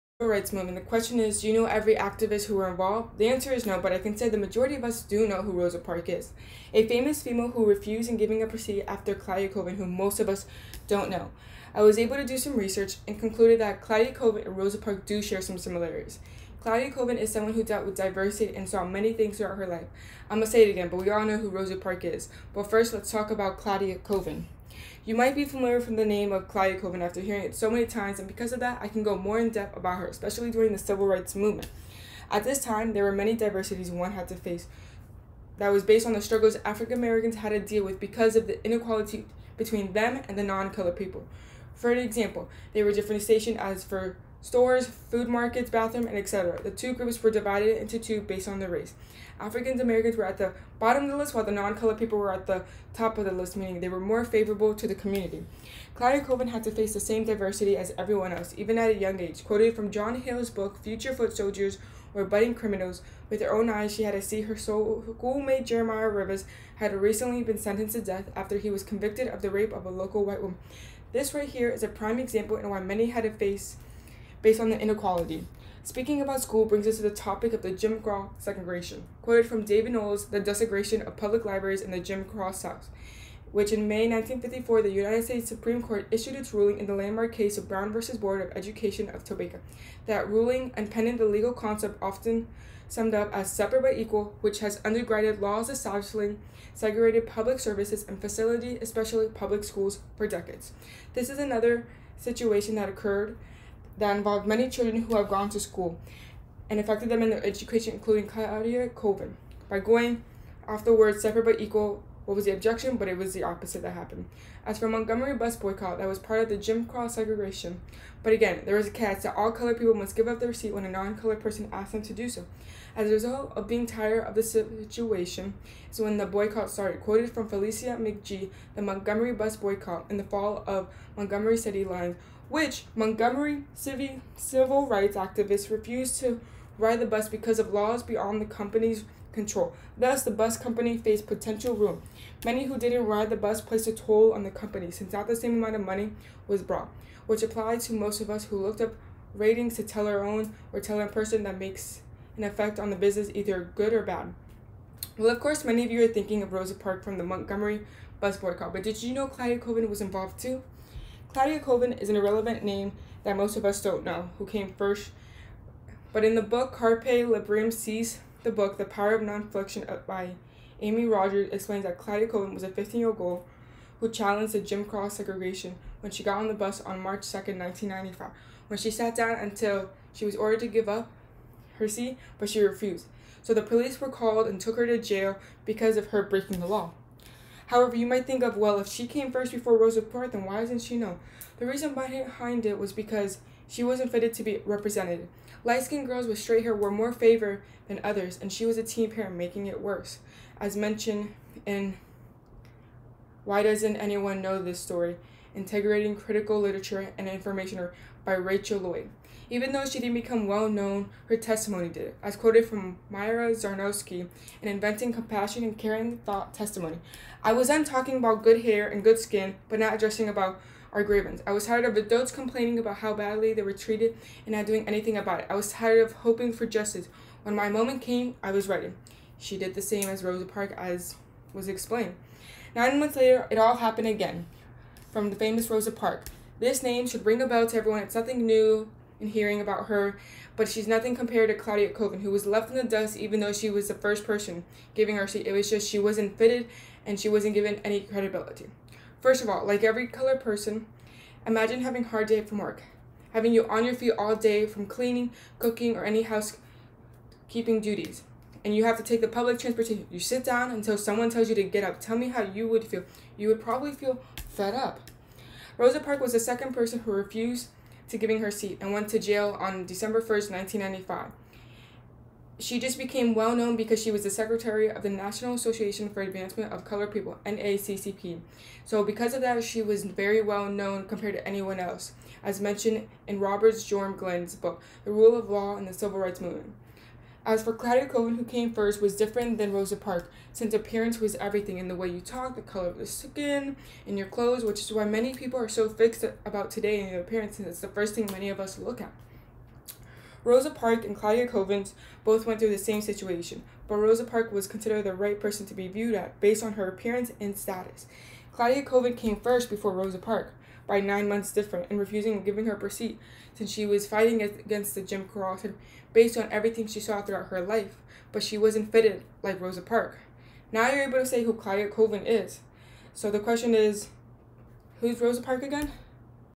Who's Claudette Colvin?
my informative speech about Claudette Colvin for my Public Speaking course